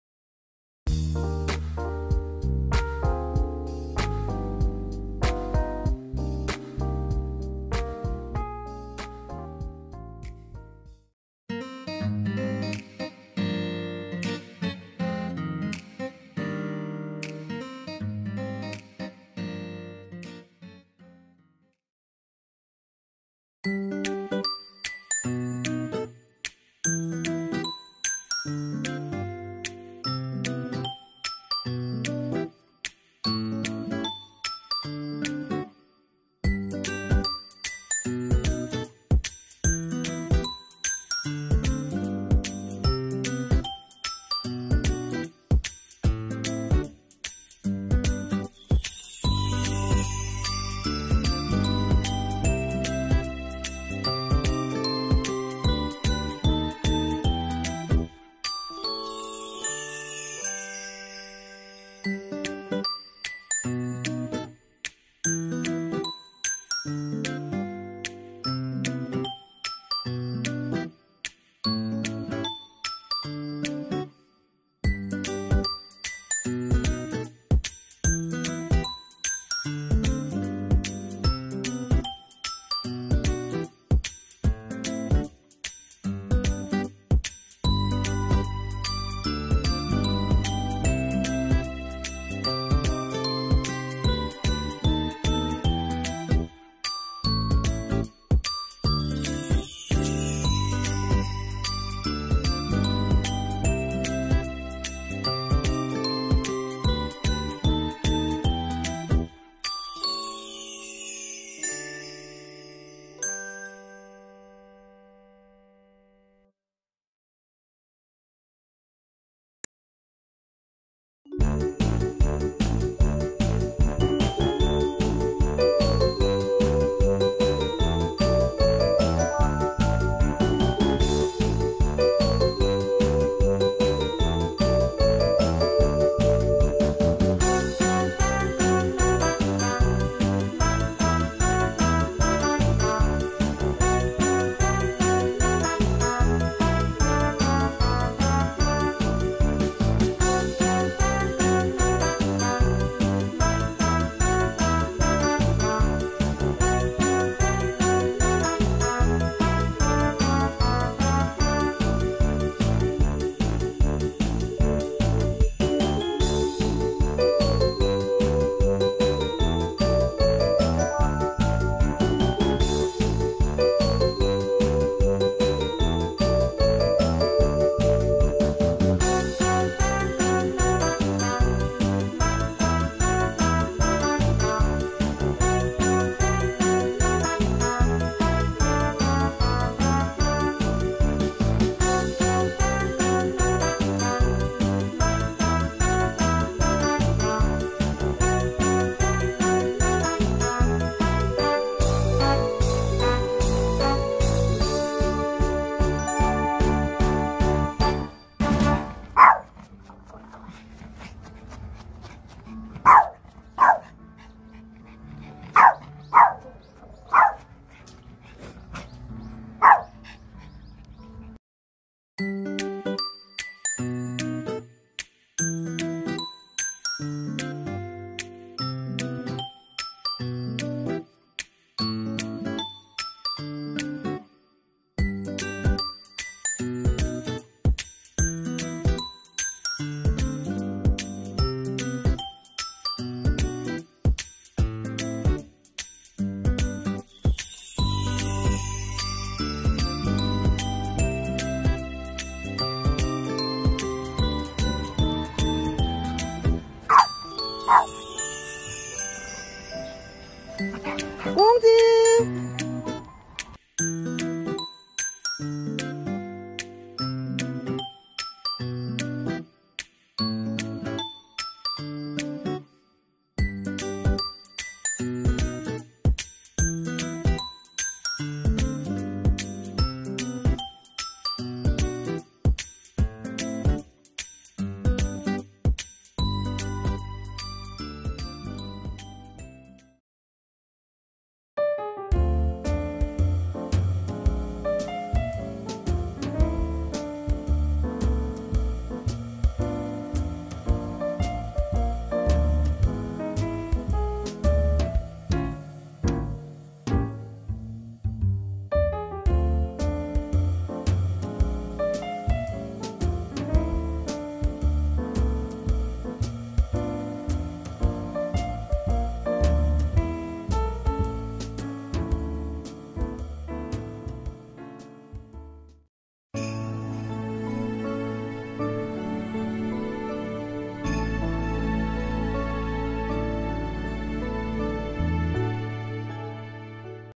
양주 애견카페 점령한 포메라니안과 미니비숑 형제|경기도 양주|퍼피킹덤
지난 주말 경기도 양주에 위치한
애견카페에 다녀왔어요
즐겁게 뛰노는 꽁희 꽁지 예쁘게 봐주세요
Pomeranian and Bichon Frise who went to the dog playground.
ドッグカフェ 遊びに行った ポメラニアン ビション兄弟

Subscriptions and likes can be a lot of power and love.
購読、いいね。 お願いします。ありがとうございます。

이름 : 김꽁희 Kkonghee
생일 : 2018년 4월 9일 3살
성별 : 남자
견종 : 포메라니안

이름 : 김꽁지 Kkongji
생일 : 2019년 10월 15일 2살
성별 : 남자
견종 : 비숑프리제

Kkonghee Kkongji Bromance